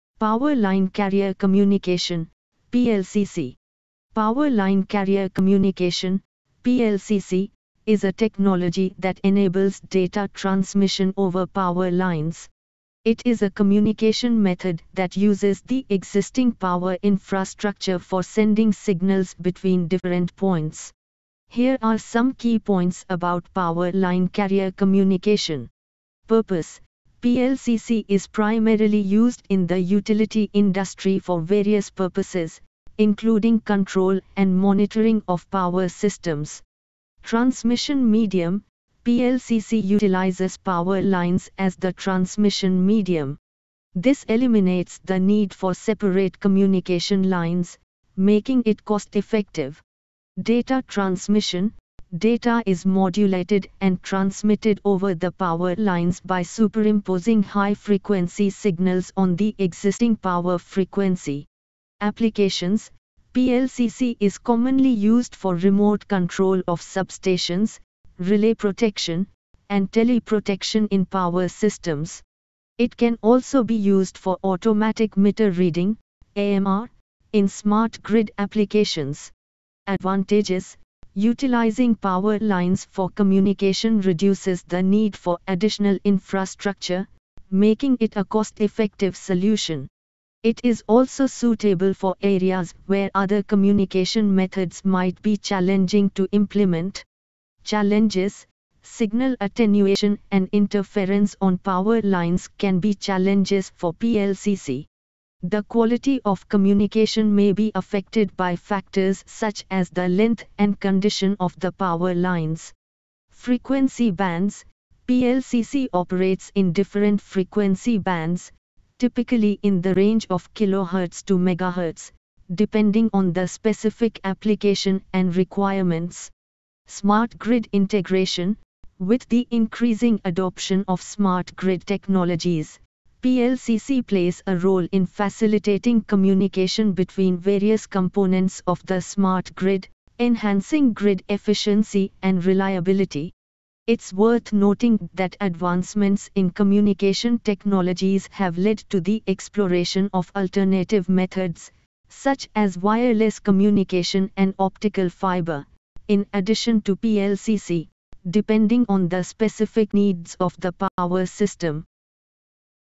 Power Line Carrier Communication PLCC Part - 2
Power Line Carrier Communication (PLCC)
Power Line Carrier Communication (PLCC) is a technology that enables data transmission over power lines. It is a communication method that uses the existing power infrastructure for sending signals between different points. Here are some key points about Power Line Carrier Communication:
Purpose: PLCC is primarily used in the utility industry for various purposes, including control and monitoring of power systems.
Transmission Medium: PLCC utilizes power lines as the transmission medium. This eliminates the need for separate communication lines, making it cost-effective.
Data Transmission: Data is modulated and transmitted over the power lines by superimposing high-frequency signals on the existing power frequency.
Applications: PLCC is commonly used for remote control of substations, relay protection, and tele-protection in power systems. It can also be used for automatic meter reading (AMR) in smart grid applications.
Advantages: Utilizing power lines for communication reduces the need for additional infrastructure, making it a cost-effective solution. It is also suitable for areas where other communication methods might be challenging to implement.
Challenges: Signal attenuation and interference on power lines can be challenges for PLCC. The quality of communication may be affected by factors such as the length and condition of the power lines.
Frequency Bands: PLCC operates in different frequency bands, typically in the range of kilohertz to megahertz, depending on the specific application and requirements.
Smart Grid Integration: With the increasing adoption of smart grid technologies, PLCC plays a role in facilitating communication between various components of the smart grid, enhancing grid efficiency and reliability.
It's worth noting that advancements in communication technologies have led to the exploration of alternative methods, such as wireless communication and optical fiber, in addition to PLCC, depending on the specific needs of the power system.